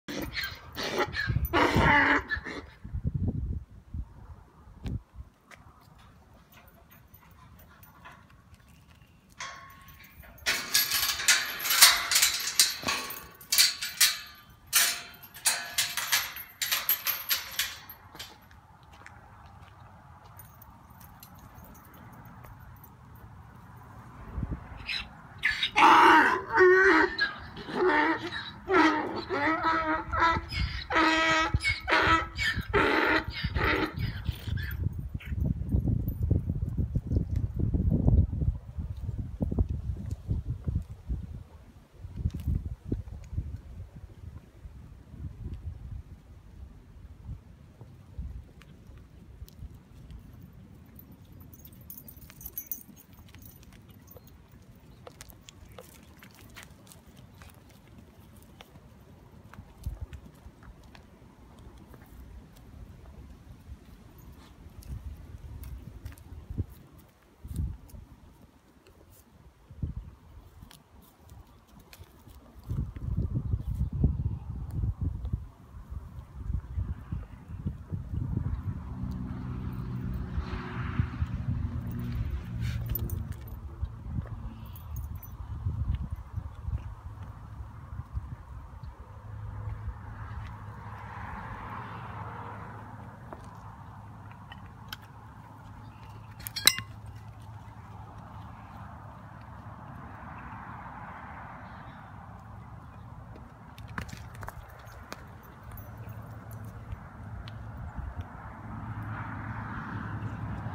Donkey Spring Fever
Lulu and Fanny from bedlam farm having some fun.